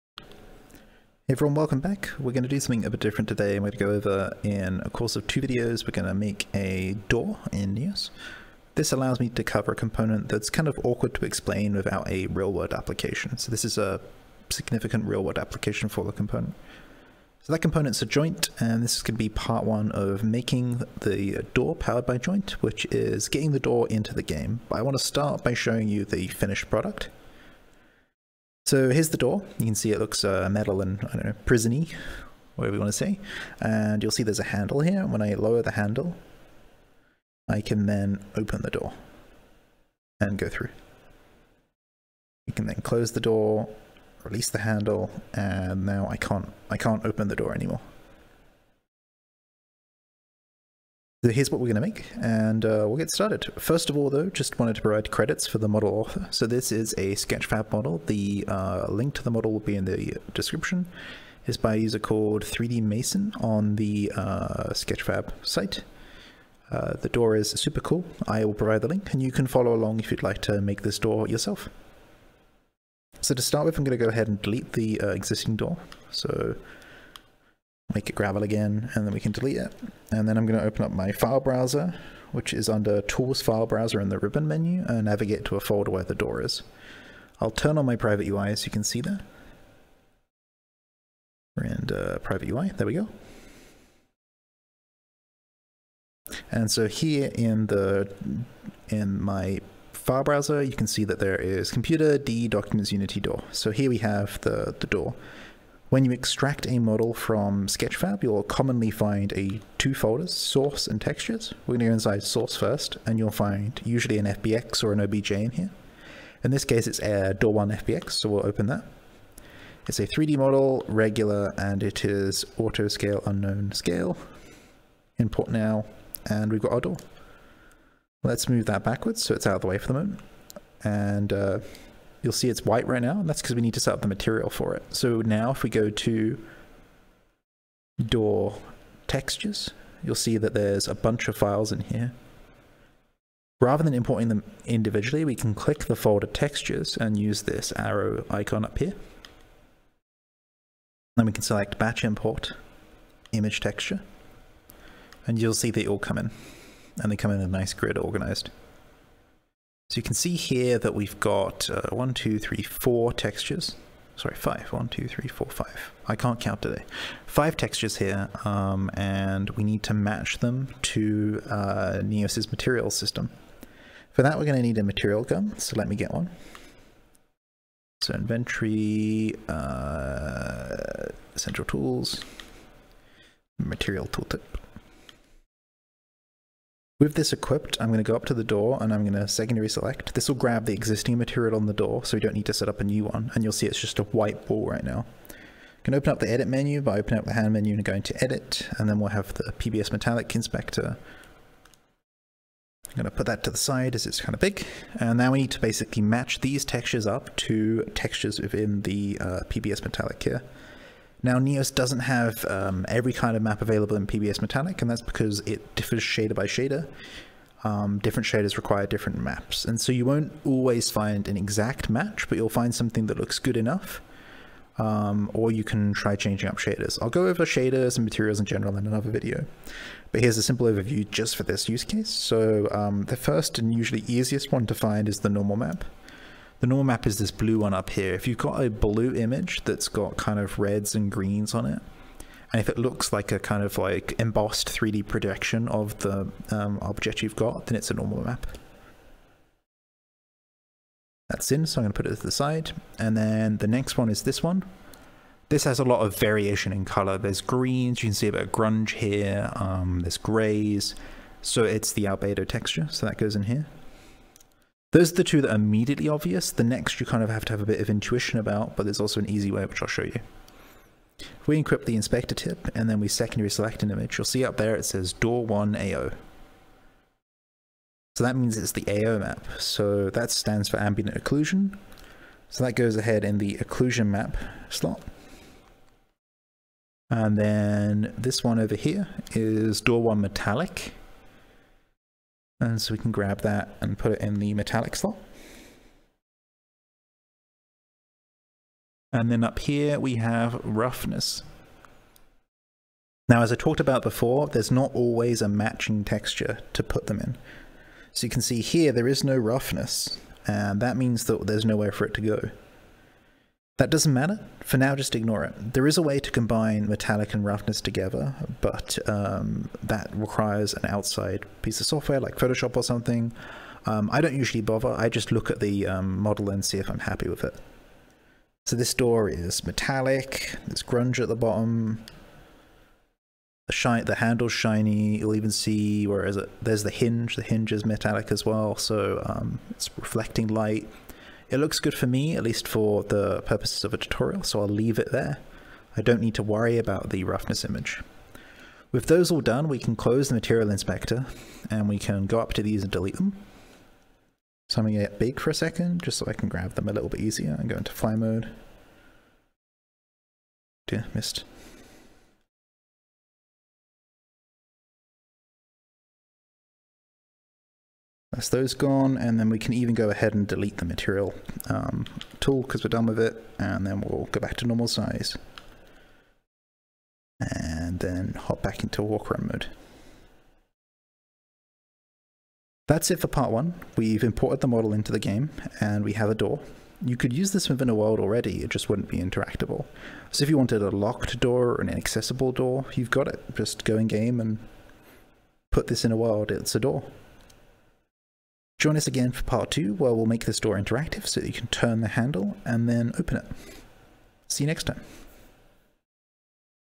OLD: Neos VR Tutorial: Using Joint to make A Door Pt. 1
In the first part of this door making tutorial we'll import the model and get its material setup.

Full credits for the model go to 3DMaesen on Sketchfab.